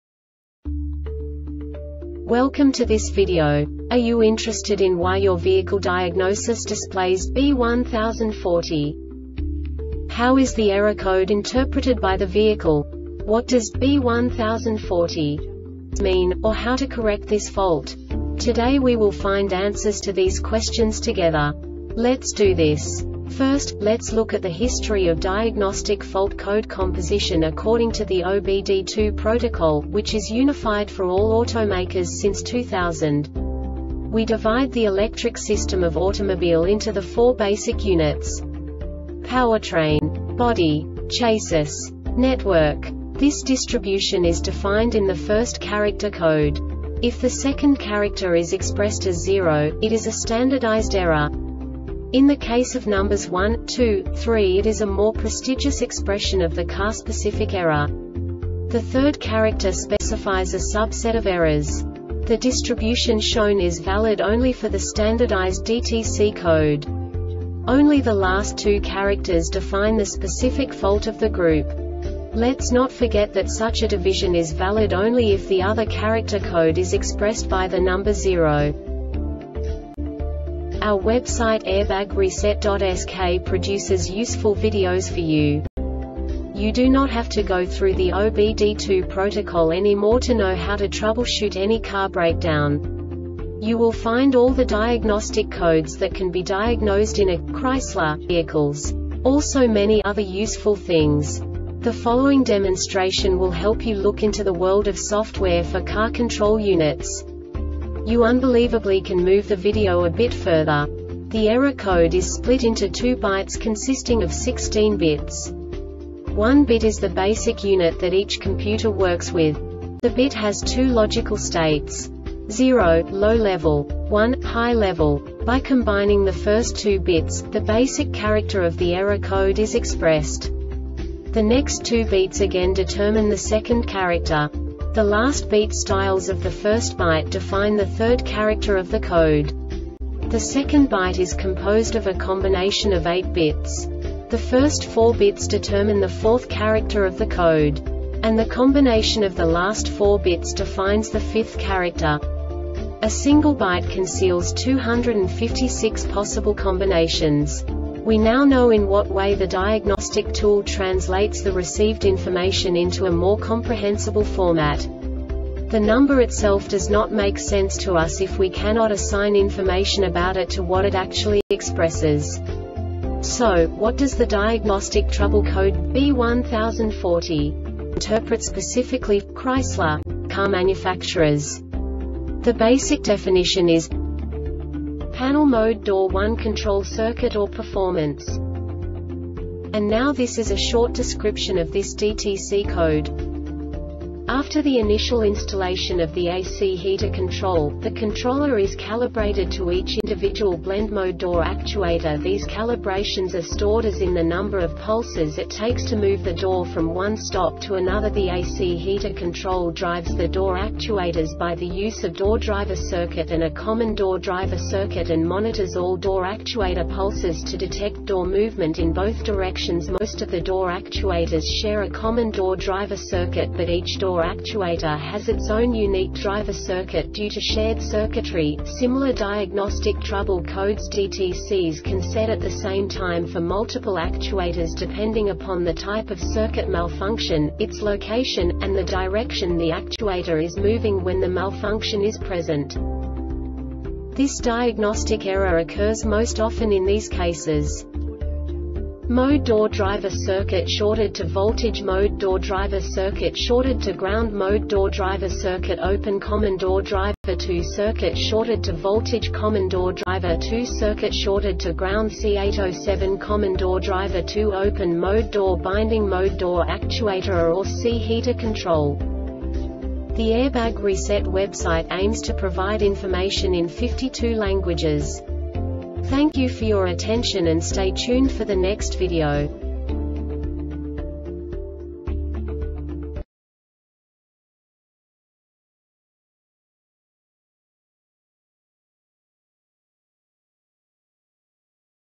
DTC Chrysler B1040 Short Explanation
Contents:
0:21 Basic DTC analysis according to OBD2 protocol standard.
1:48 Insight into programming
2:58 A diagnostically understandable description of the Diagnostic Trouble Code
3:05 Basic error definition

Make
Chrysler
Code
B1040
Definition
Panel Mode Door 1 Control Circuit or Performance
Description
After the initial installation of the A or C Heater Control, the controller is calibrated to each individual blend or mode door actuator These calibrations are stored as in the number of pulses it takes to move the door from one stop to another The A or C Heater Control drives the Door Actuators by the use of Door Driver circuit and a Common Door Driver circuit and monitors all door actuator pulses to detect door movement in both directions Most of the door actuators share a common door driver circuit but each door actuator has its own unique driver circuit Due to shared circuitry, similar Diagnostic trouble Codes DTCs can set at the same time for multiple actuators depending upon the type of circuit malfunction, its location, and the direction the actuator is moving when the malfunction is present